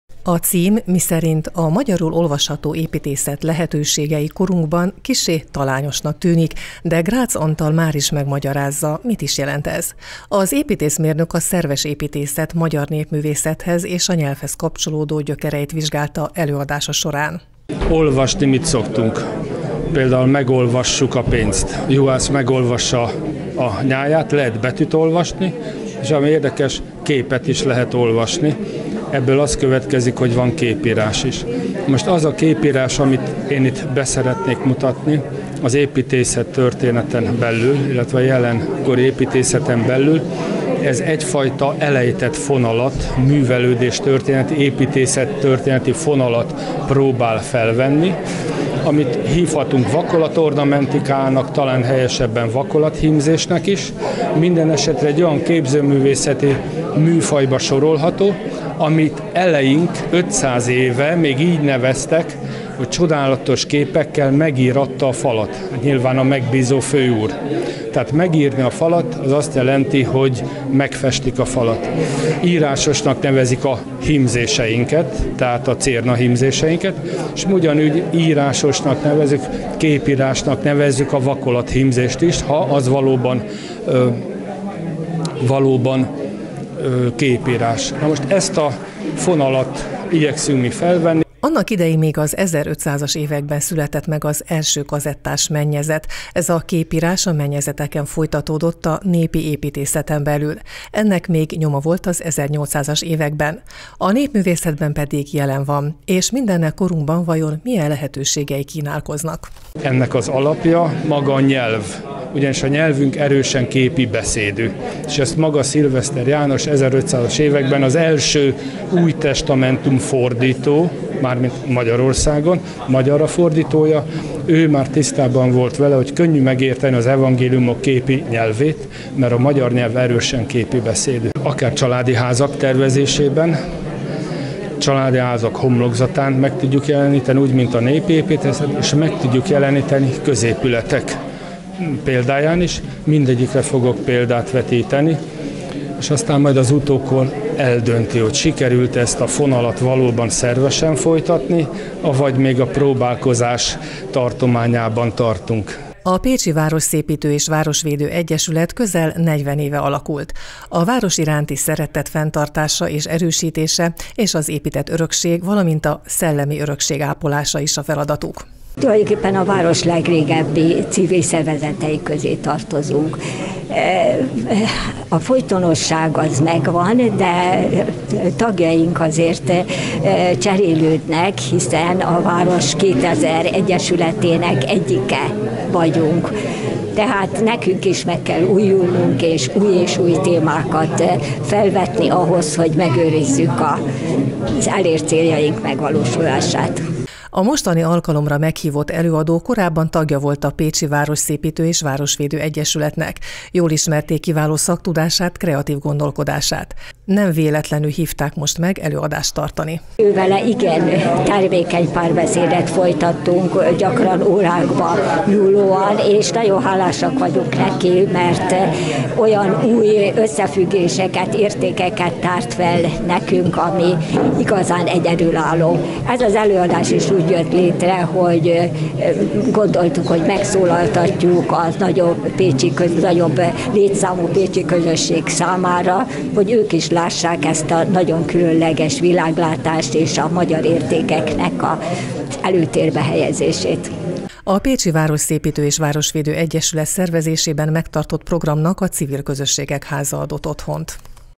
At építészet lehetőségei korunkban
"A magyarul olvasható építészet lehetőségei korunkban" címmel Grátz Antal építészmérnök tartott előadást a Pécsen a Civil Közösségek Házában. A program a Pécsi Városszépítő és Városvédő Egyesület szervezésében jött létre.